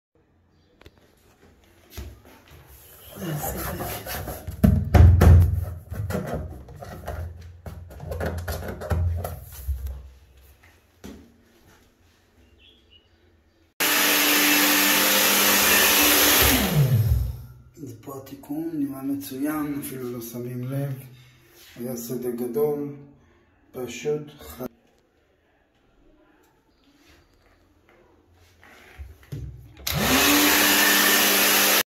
בלעדי בעולם תיקון שבר סדק באמבטיה אקרילית 050-6496875
תיקון שסר באמבטיה אקרילית באפוקסי